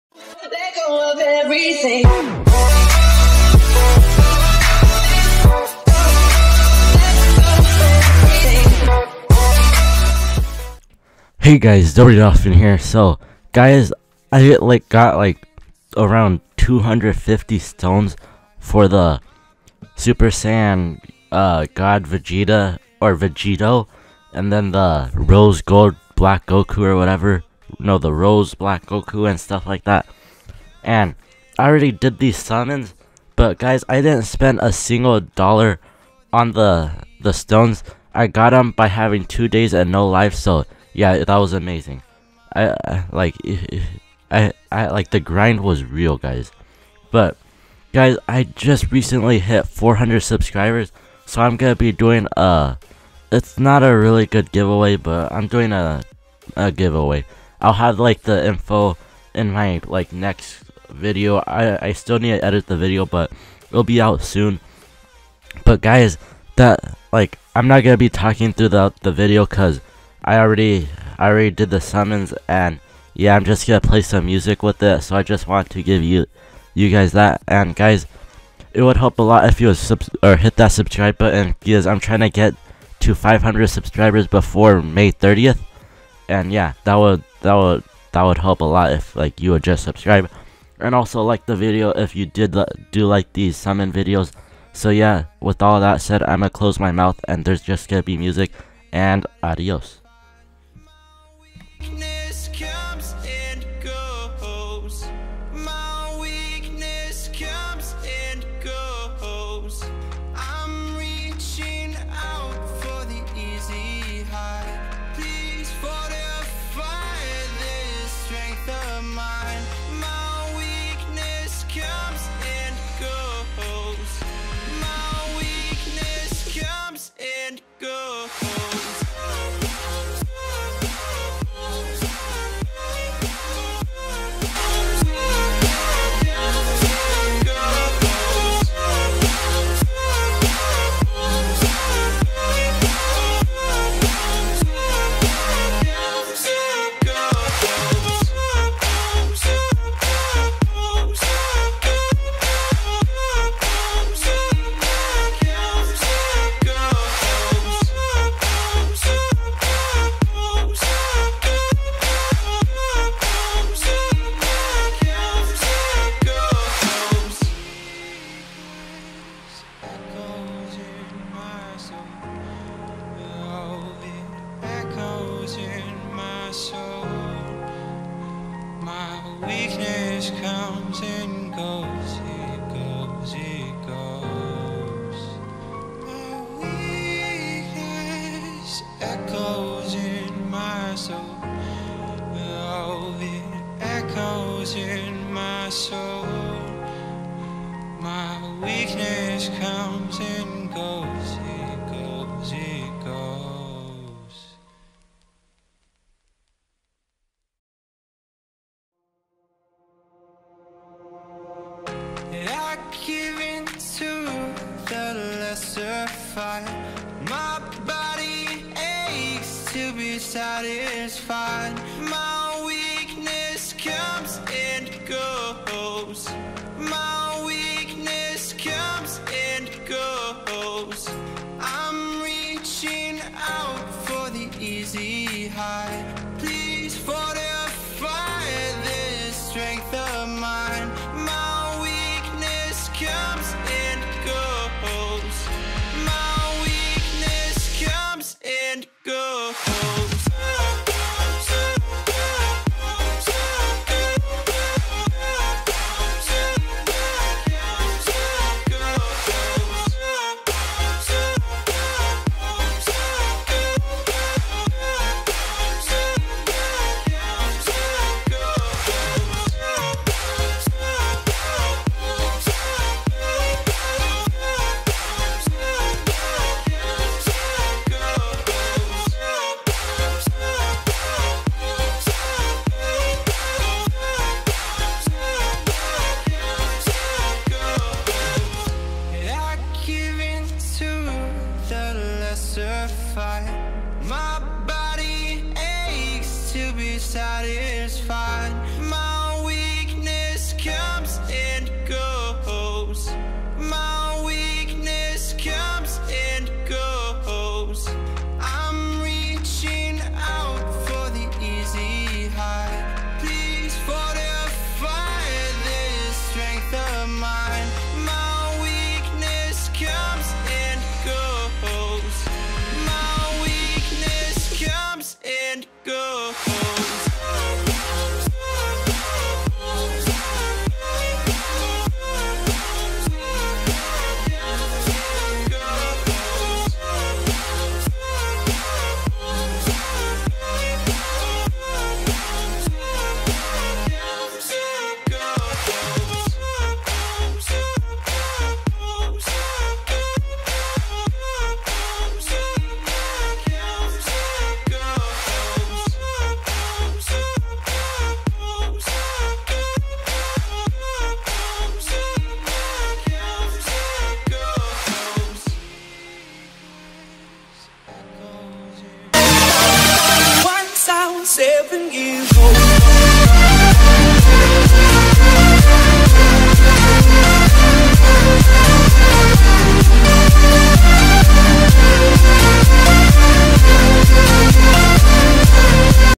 250 STONES SUMMON ON SSGSS VEGITO AND ROSE BLACK GOKU BANNER
ALSO IF U R READING THE DESCRIPTION THEN U WILL GET AN SSR IN A MULTI SUMMON TODAY IF U LIKE THE VIDEO!